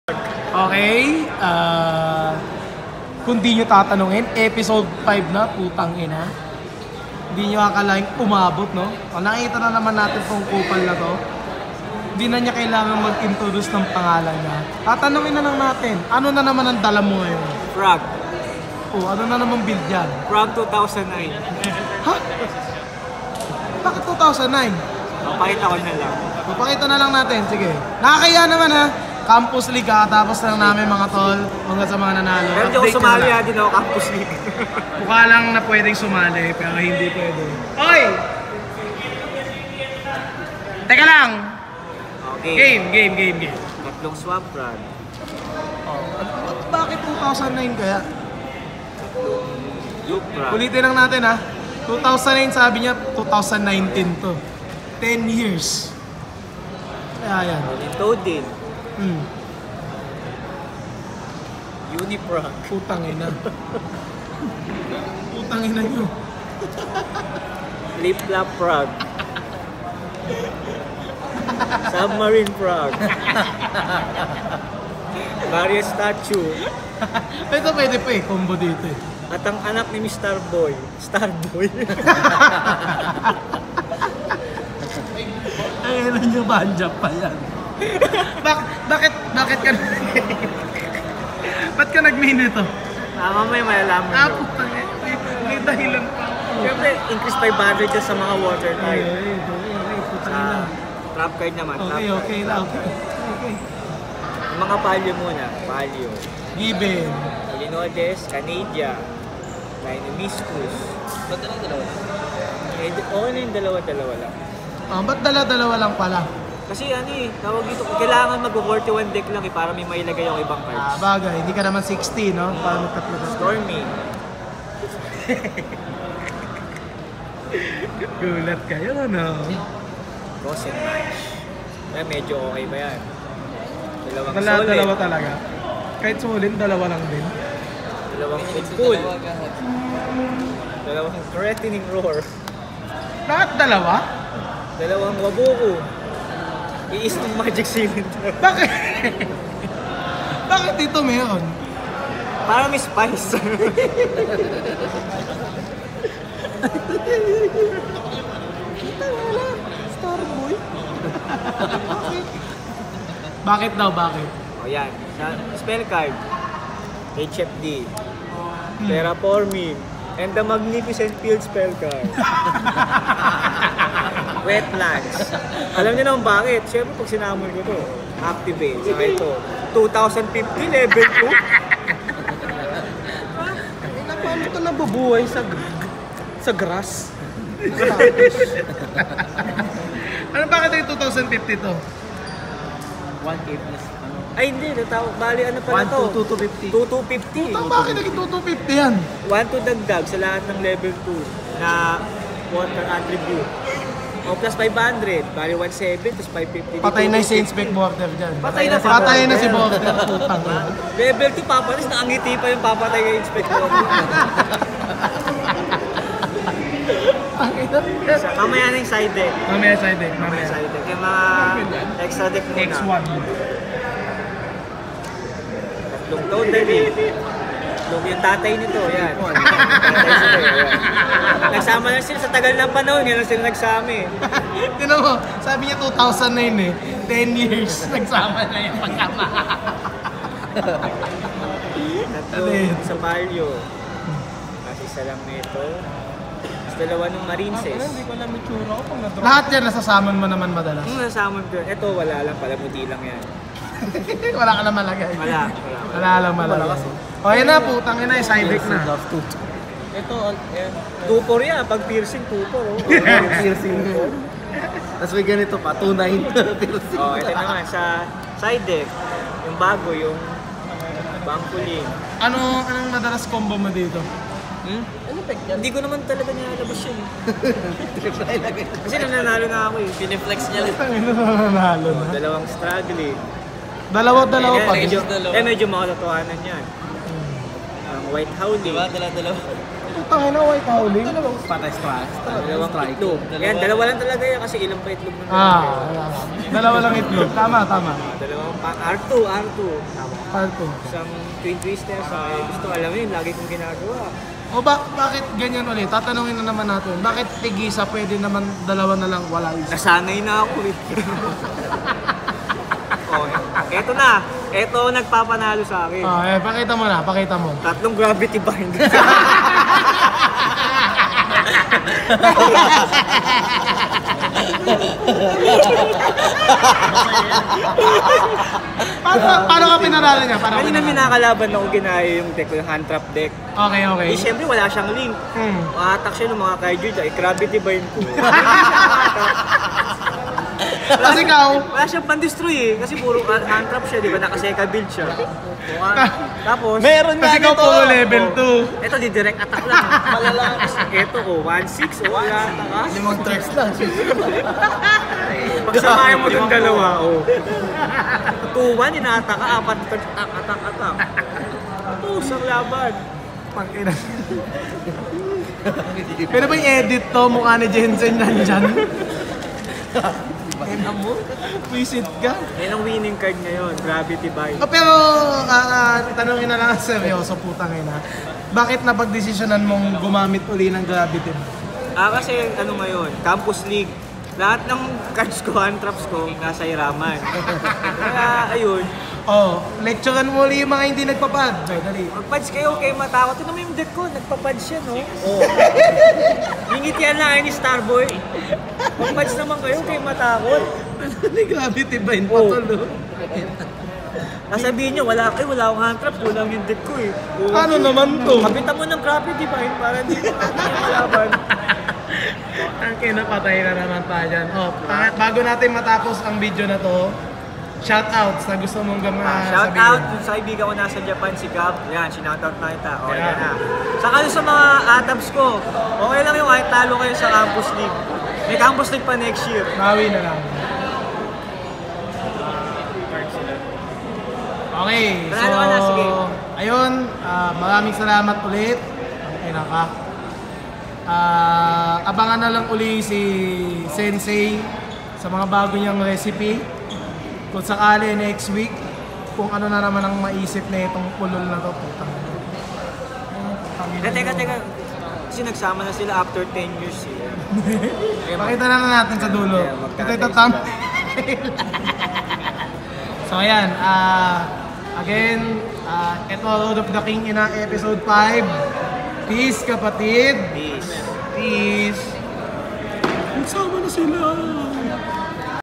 Yu-Gi-Oh! ROAD OF DA KING INA Deck Profile Series - Xavier Jensen Ignacio (Paleo Star Boy Frog)
ROAD OF DA KING INA

Our Da King Ina sensei Xavier Jensen Ignacio is back and he’s showing his “resourcefulness” just to play in ranking tournaments even if majority of his cards were borrowed for the recent Campus League 2019. Here’s the update for his “Paleo STAR BOY Frog” with his CURVEBALL SIDEBOARD!